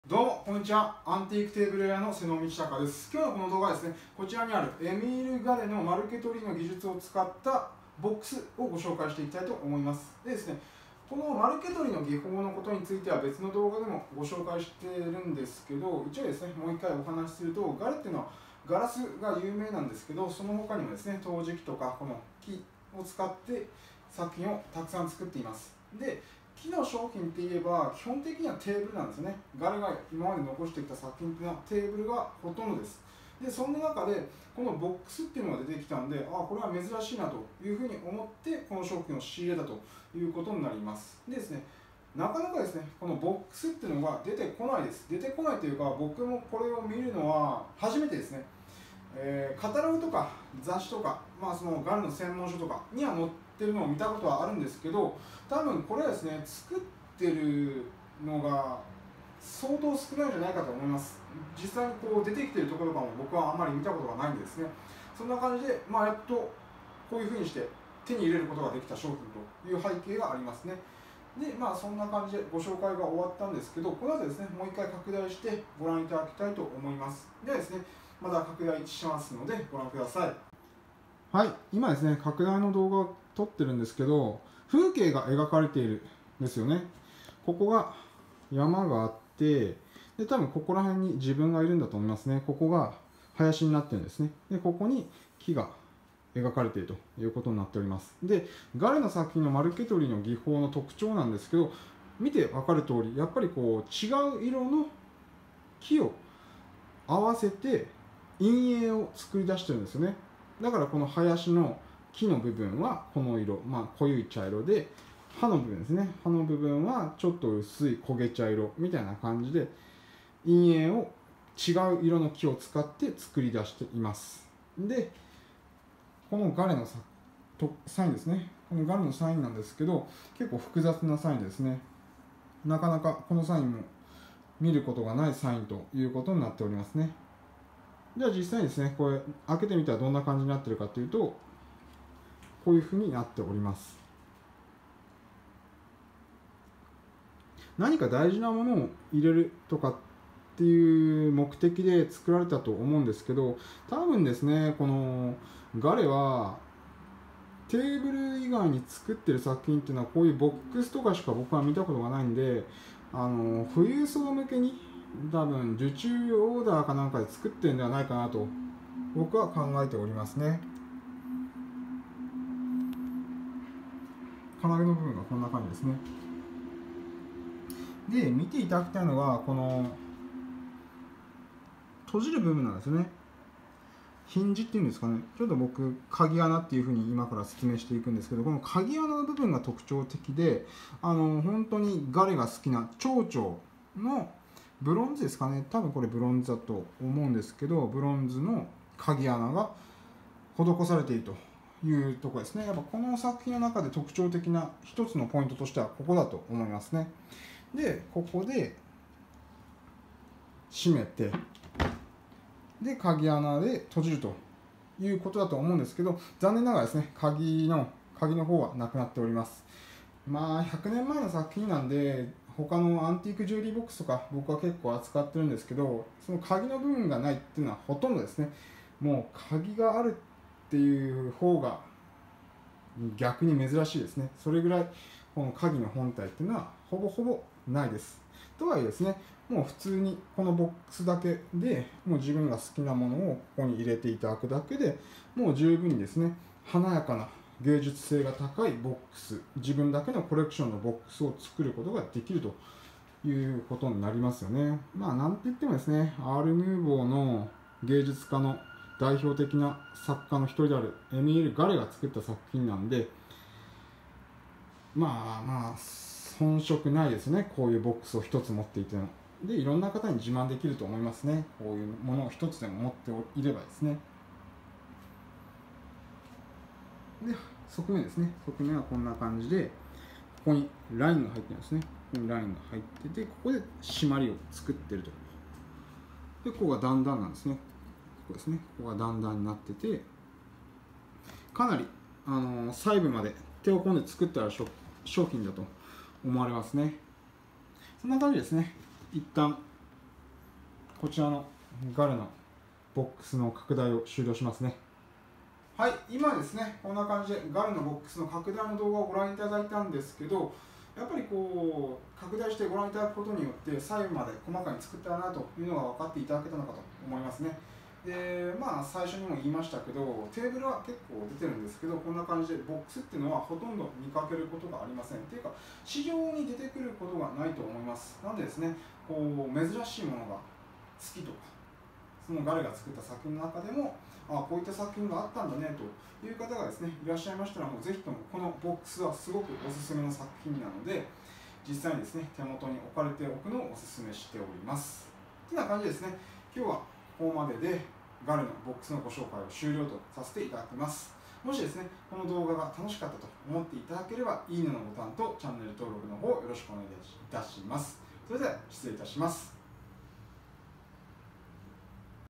エミールガレの最高傑作！ マルケトリ技法を使ったボックス（木製ウッド作品）
エミールガレ 初期作品 マルケトリ技法を使ったボックス（木製ウッド作品）になります。
ガレがガラスの作品を作り出す前に作っていた初期の作品になります。
マルケトリ技法とは、異なる色の木をつなぎ合わせて陰影を作り出す技法で奥行きが出るのが特徴です。
今回の作品なのですが、基本的にガレは木製作品の多くはテーブルだったのですがこちらの作品は珍しくボックスになっております。
よってこれは私の予測なのですが、おそらく富裕層の方からオーダーを受けて作ったのではないかと思われます。
鍵の部分はガレが多くの作品に取り入れた蝶々のヒンジがブロンズで取り付けられており作品全体を通して見ても、最高傑作であるのが伺えます。

状態
目立つ欠け、割れ、傷なく基本的良好な状態です。

サイズ
３９cm x ２９cm
高さ
９cm

こちらの商品は私が力を入れて仕入れてる商品になりますので、お手元に置かれたら確実に満足して頂ける商品と思います。